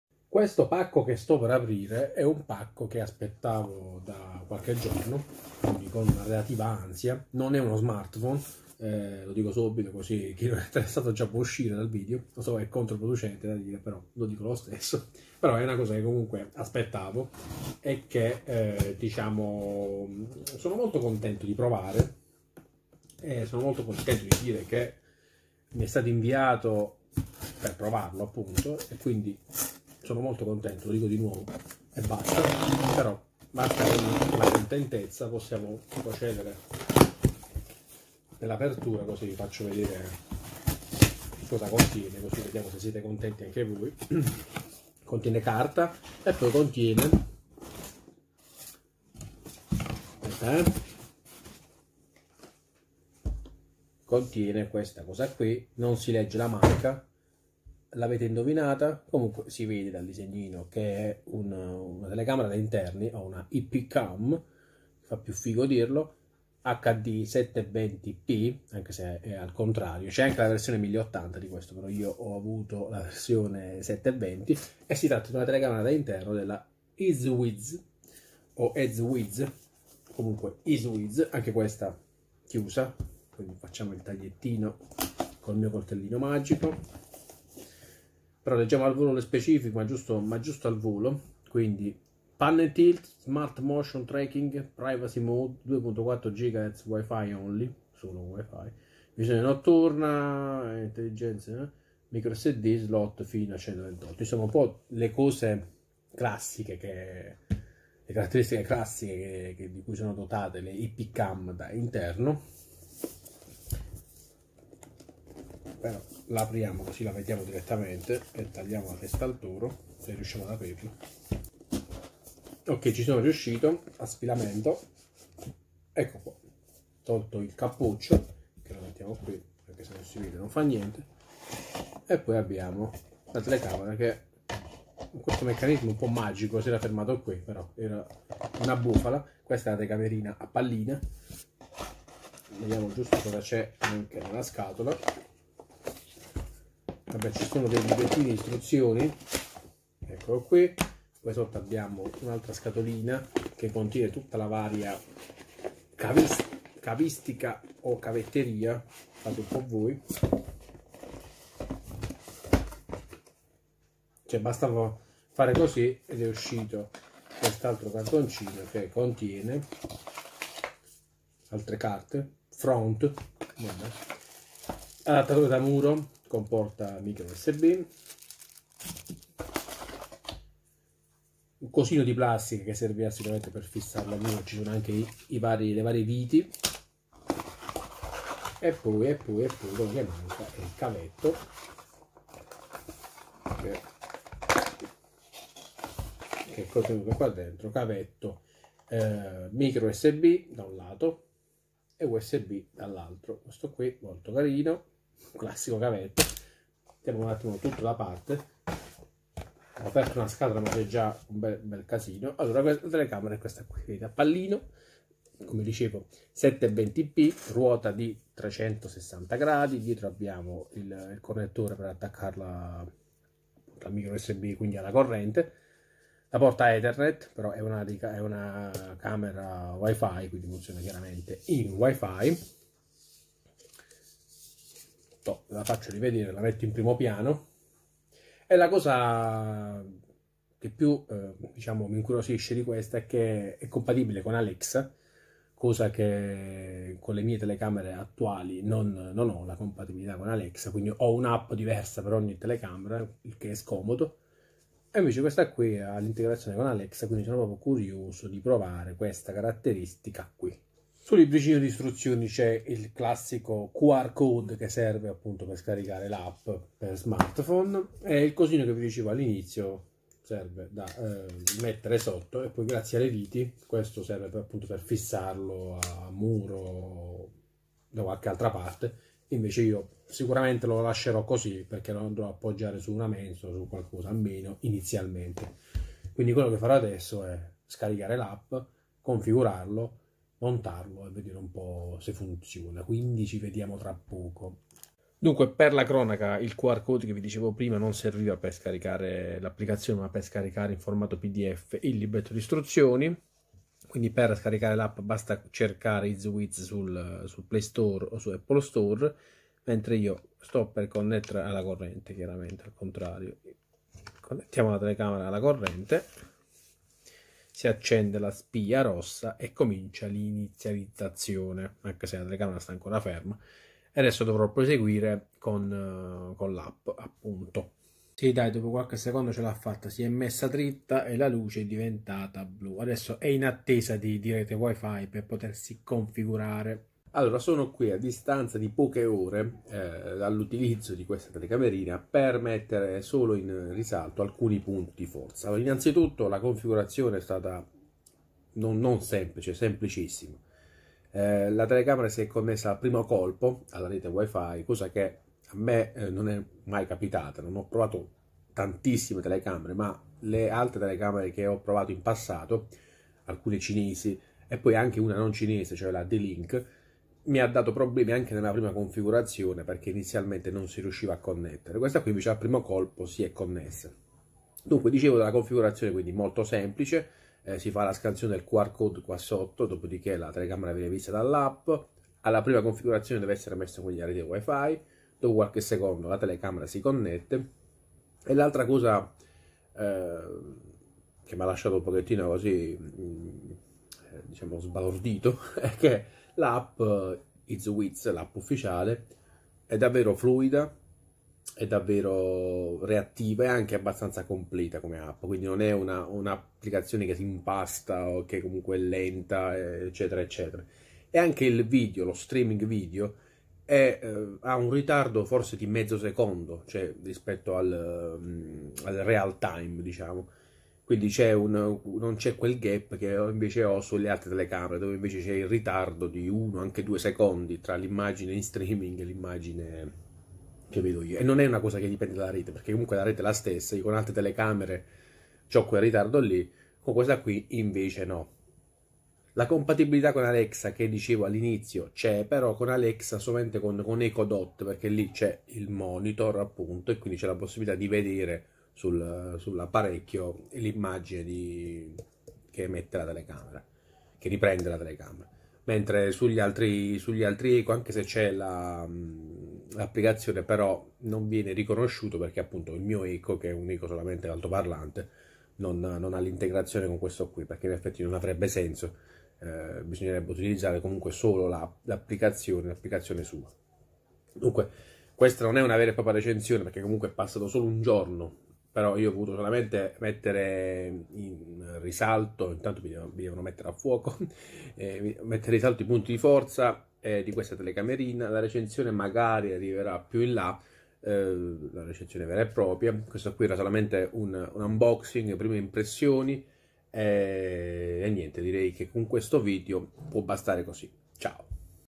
Unboxing IP cam
Unboxing e prime impressioni riguardo questa ip Cam da 720p
➡️Link Amazon